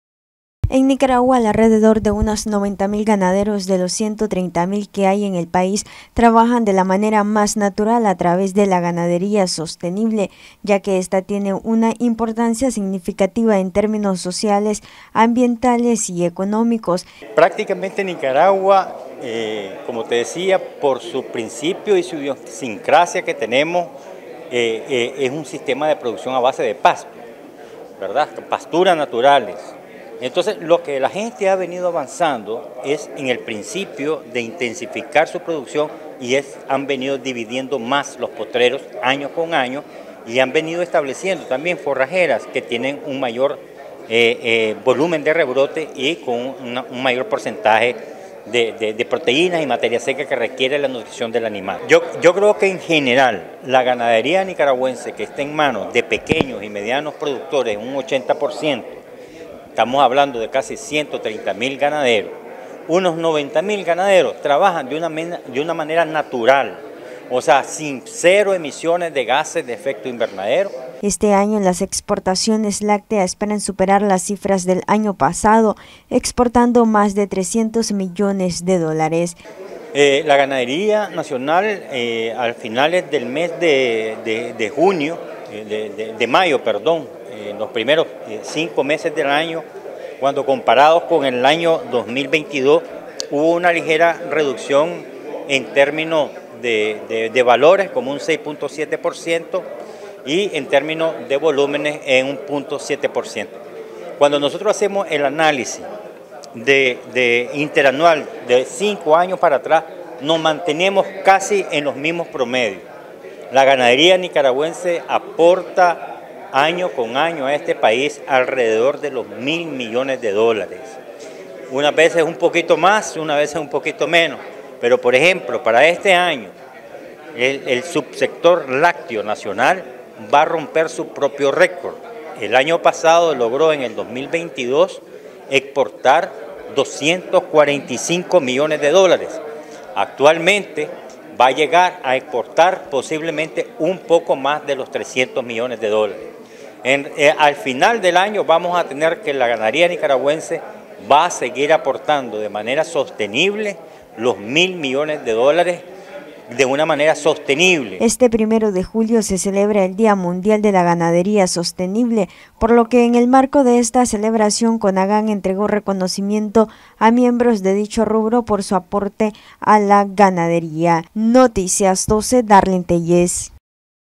Noticias12Nicaragua Este año sector lácteo espera generar 300 millones de dólares en exportaciones
Este año el sector lácteo espera generar 300 millones de dólares en exportaciones